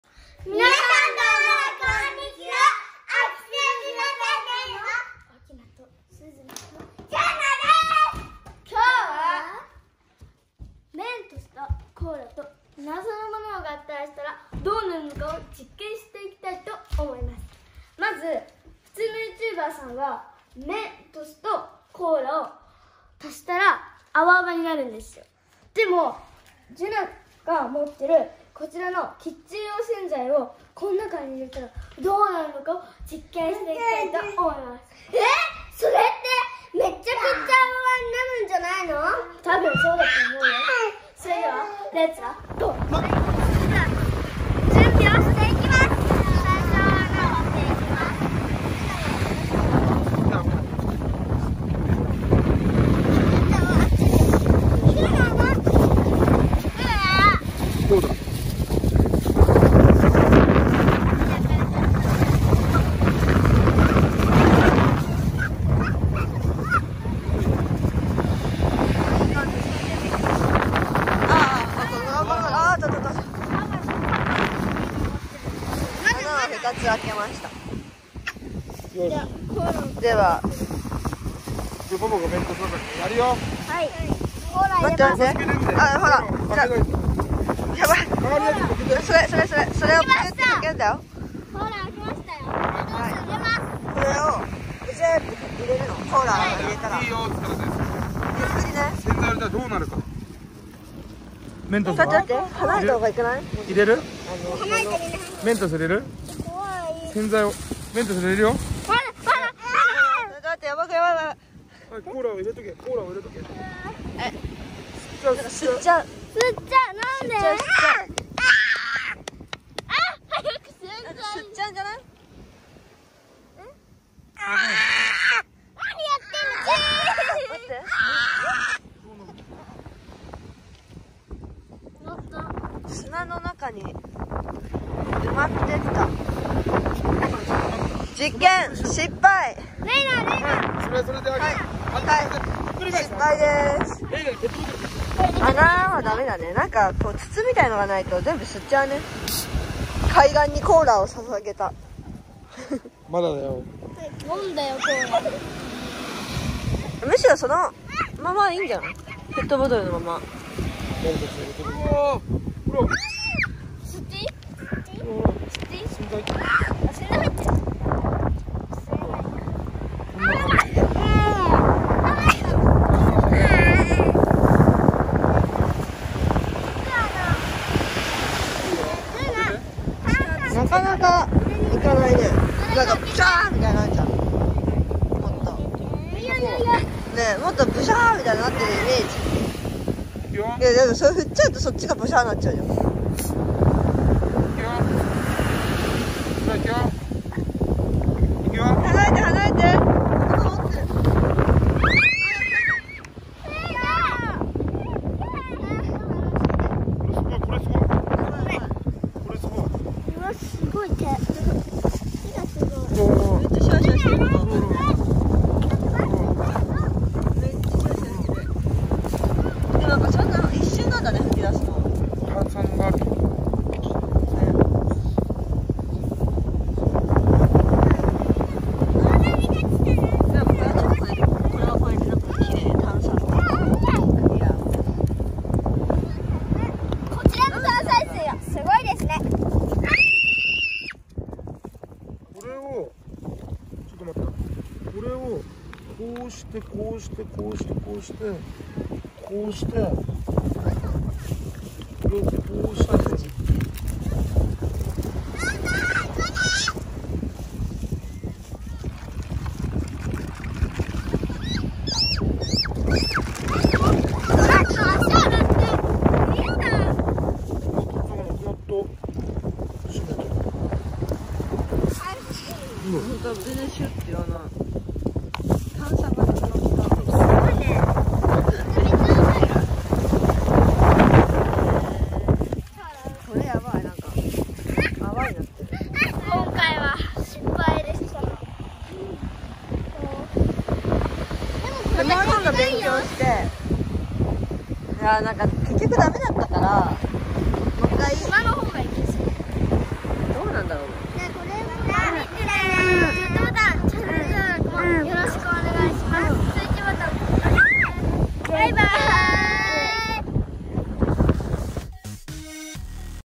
【実験】メントスコーラー🥤🚀やってみたら大惨事！？😱💦
メントスコーラー今更ながらやってみました😂‼︎
子供たちの勉強になれば！と思います。